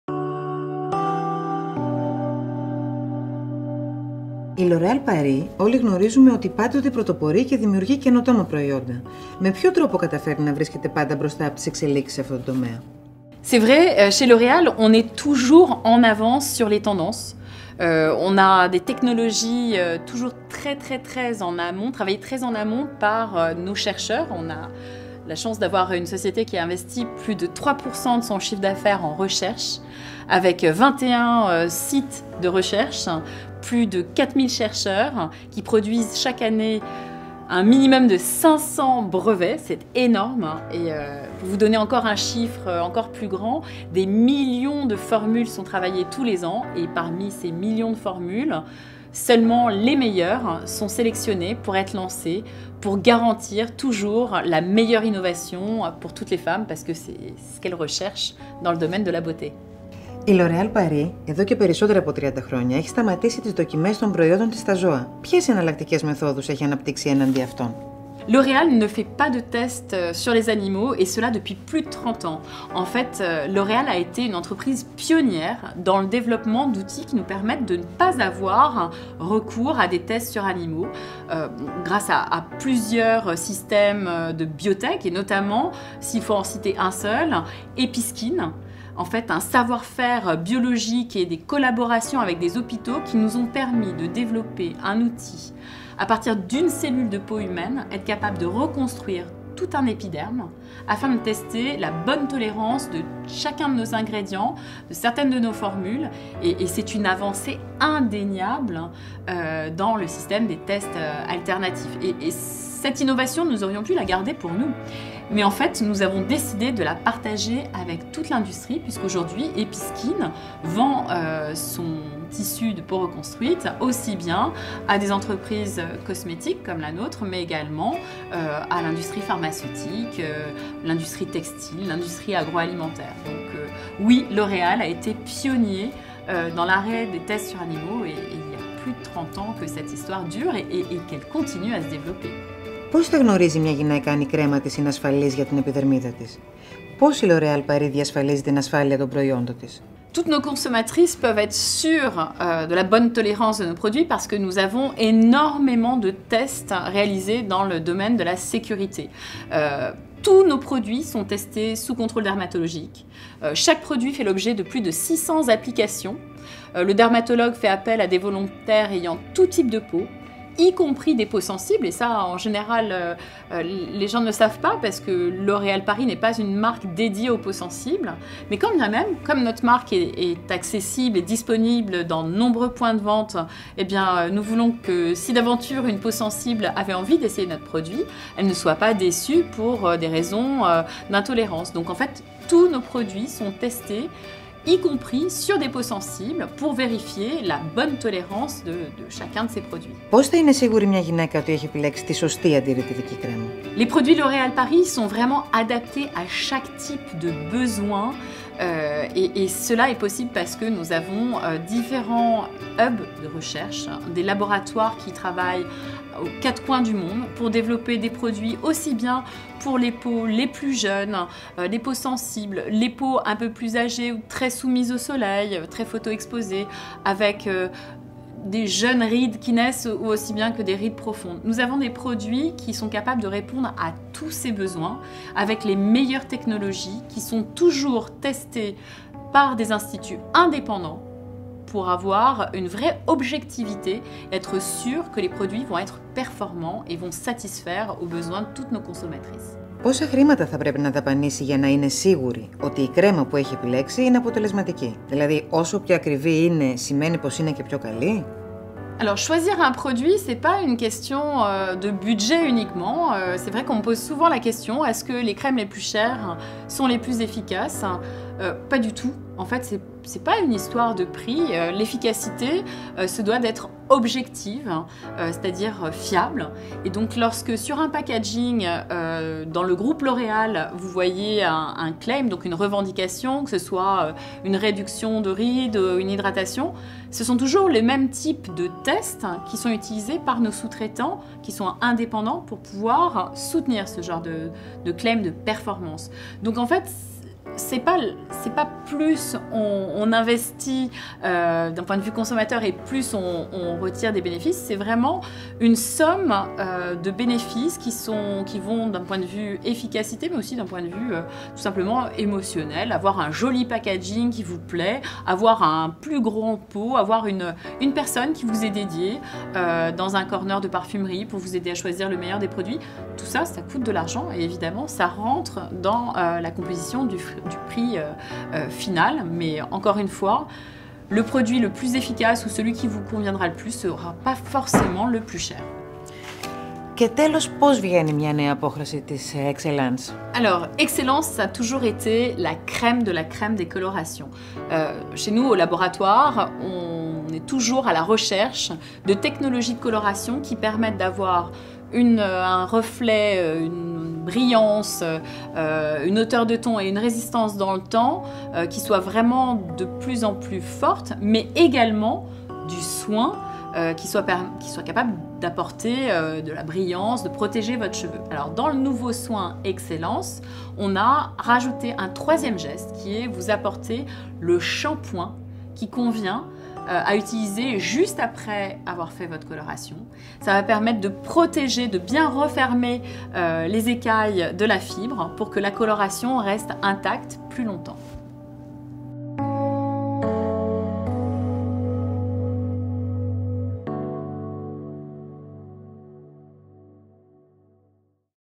L'Oréal Paris |Elisabeth Bouhadana • the interview film
directed & filmed by_ yeoryios k tantos
sound by_ yannis baxevanakis
executive produced by_ andreas c saplaouras
produced by_ slas productions /slas.gr

L'Oréal Paris Research & Innovation
feat. Eleni Menegaki & Elisabeth Bouhadana
at L'Oréal Paris Scientific Laboratories, France